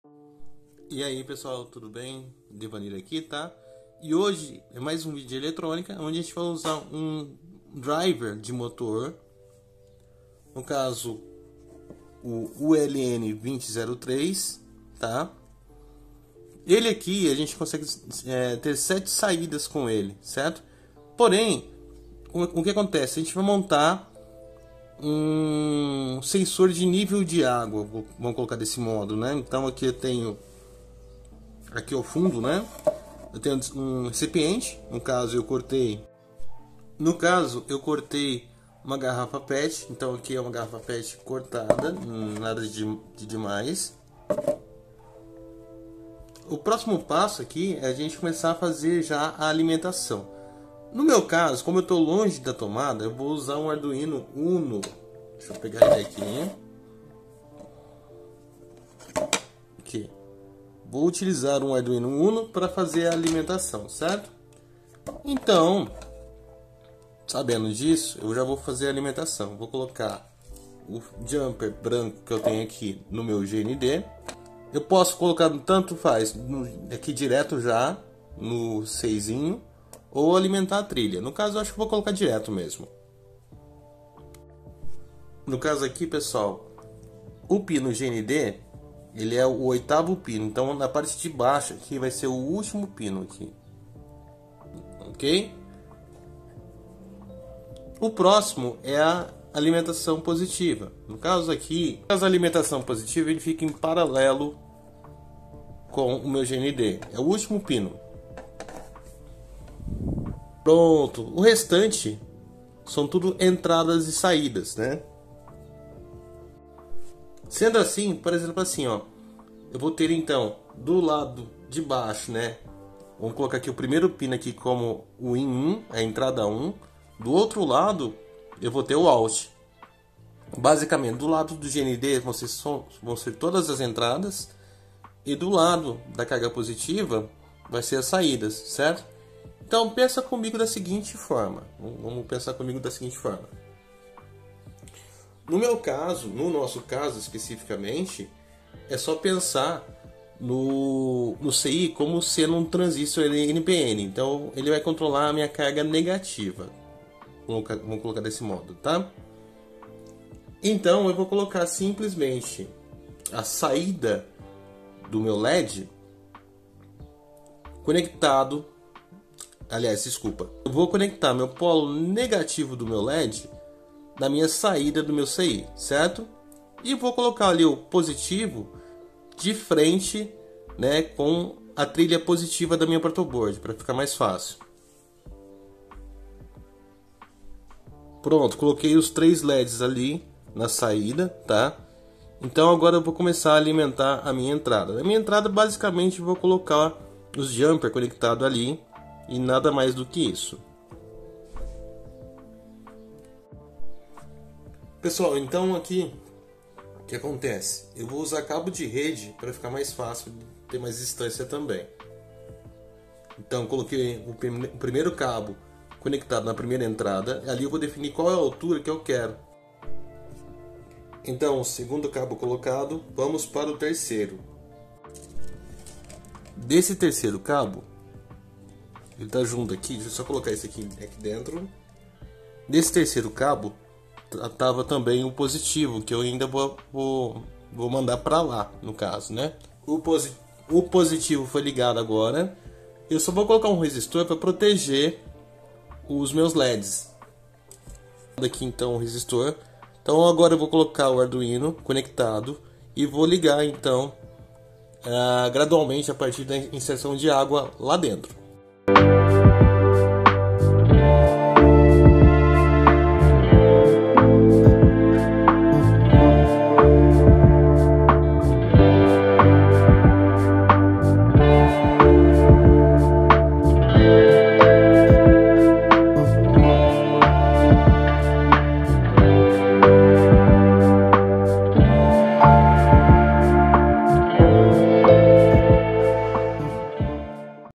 NÍVEL DE ÁGUA / WATER LEVEL [ULN2003]
Explore conosco neste vídeo fascinante um projeto prático de medição de nível de água usando o ULN2023. Vamos guiá-lo passo a passo na criação de um sensor personalizado para monitorar o nível de água em tempo real. Descubra como integrar o ULN2023 de forma eficiente, garantindo precisão e confiabilidade na medição. Este tutorial abrangente é perfeito para entusiastas de eletrônica e projetos DIY. Aprenda a construir seu próprio sistema de medição de nível de água e leve suas habilidades de automação para o próximo nível.